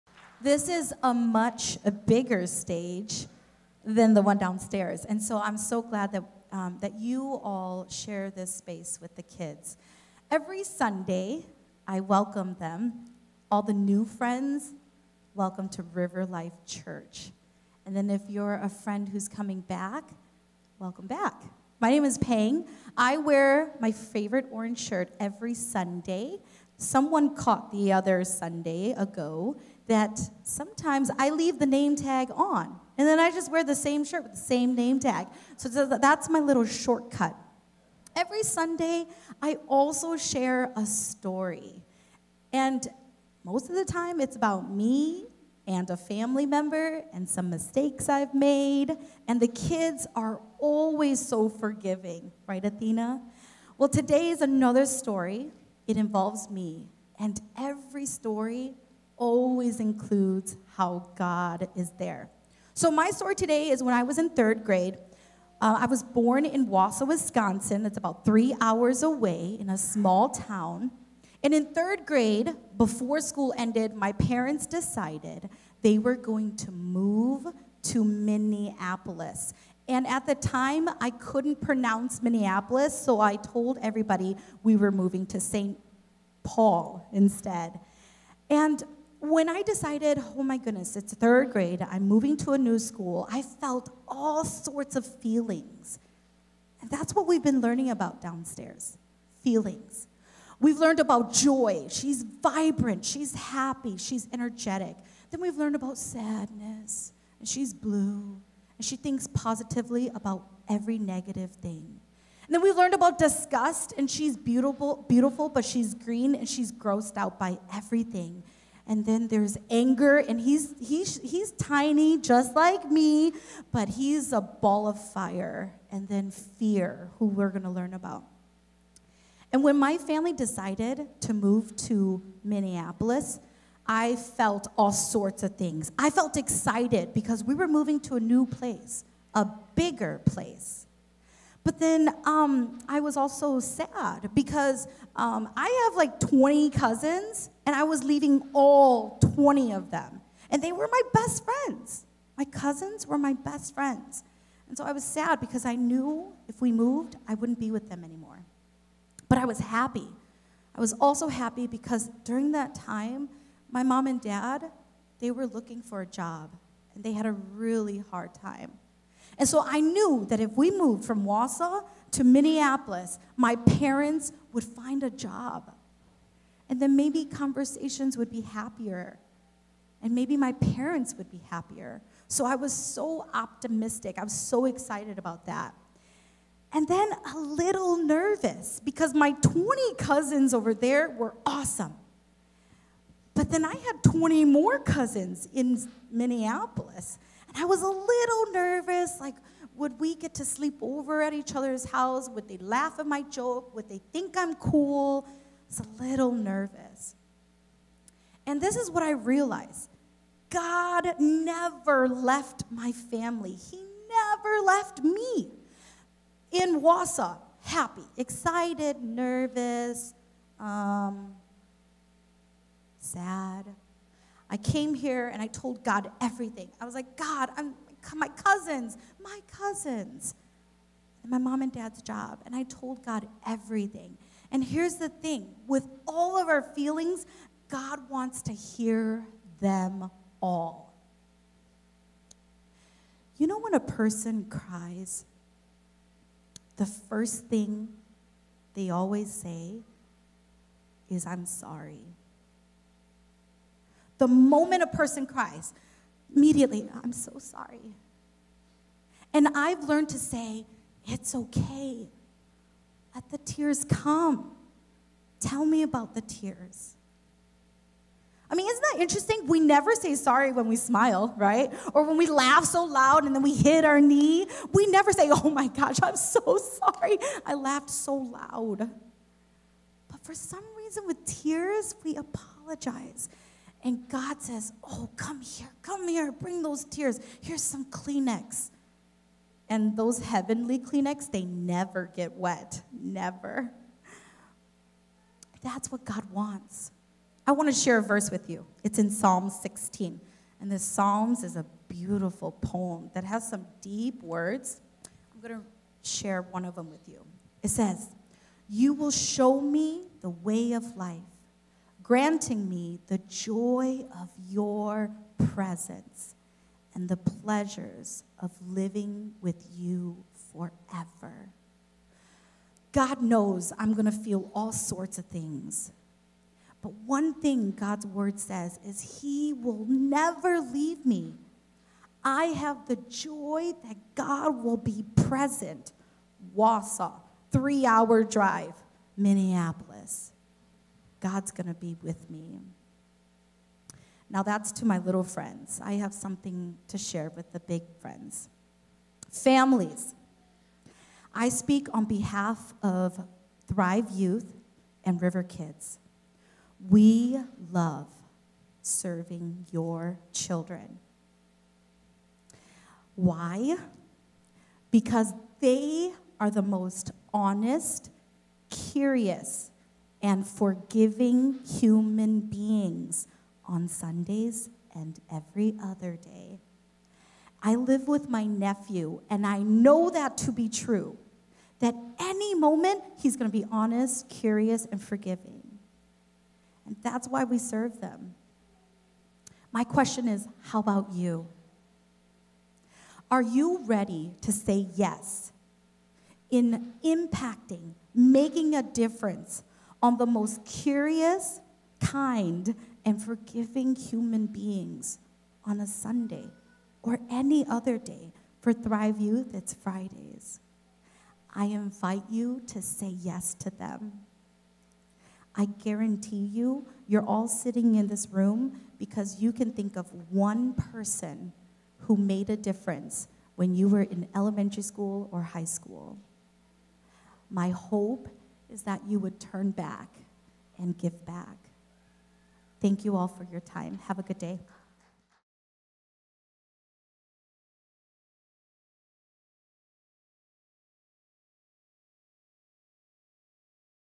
Hey Kids, God is with You in All Your Emotions | Pang Moua
During our special Family Service, Pang Moua, our Children’s Director, shared with all the children in service about all the different kinds of emotions you can experience and the important fact that God is with you in all of them. This was part of their current series on emotions based on the movie “Inside Out.”